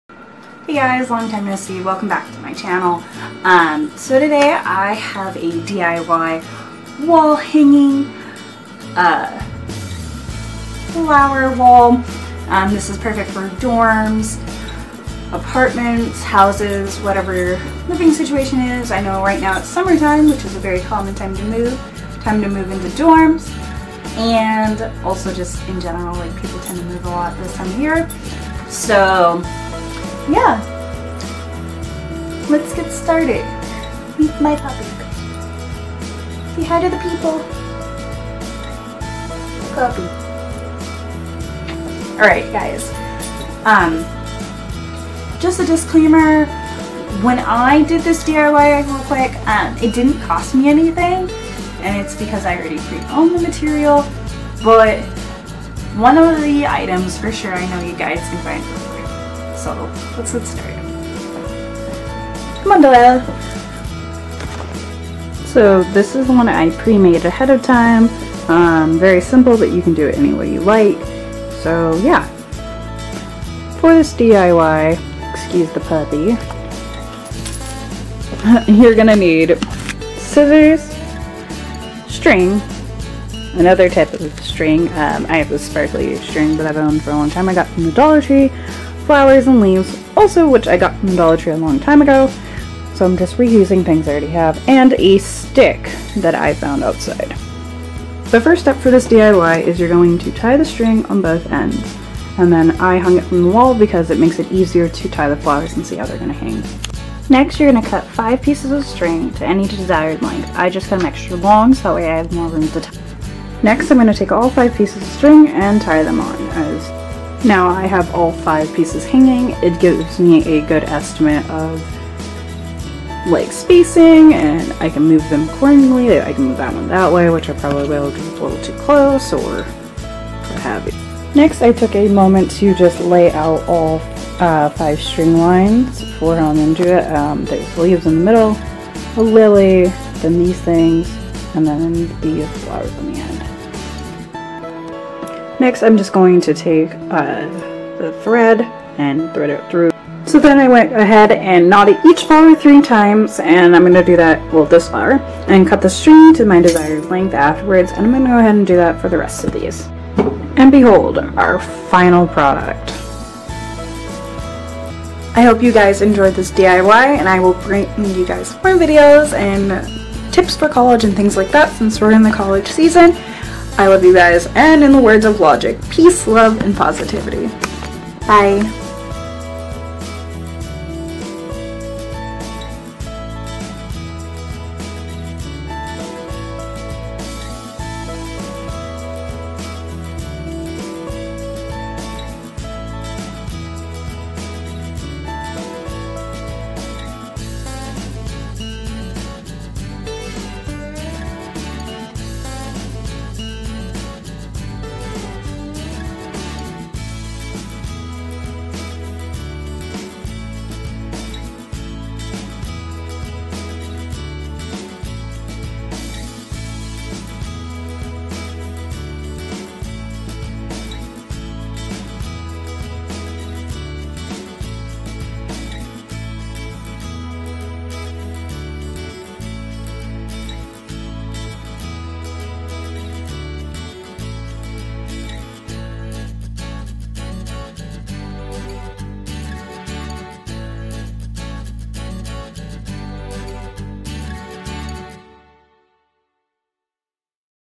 DIY Boho hanging flower wall
Tis the season of moving into dorms, apartments, and moving in general. So I figured I would make something with you guys that is great for any dorm, apartment, or what have you. It is cheap and easy